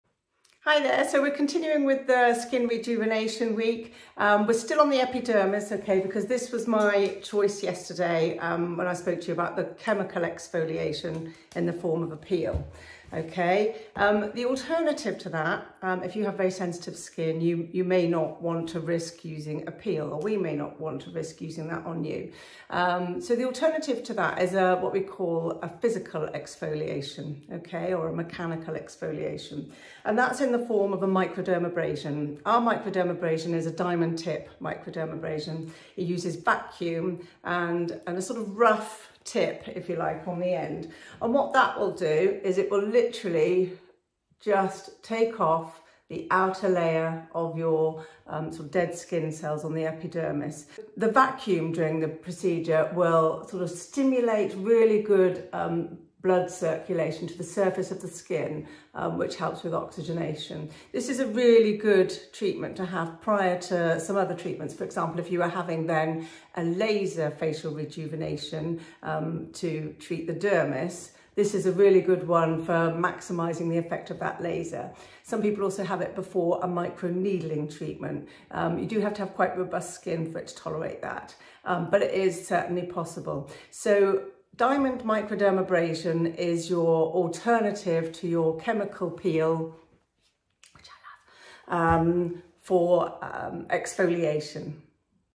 Facial Rejuvenation Series - Part 2 Diamond Microdermabrasion
Microdermabrasion is a physical exfoliation treatment - again this will clear the outer layer leaving you with a bright even skin surface.  Excellent for adding to other facial treatments such as laser or microneedling.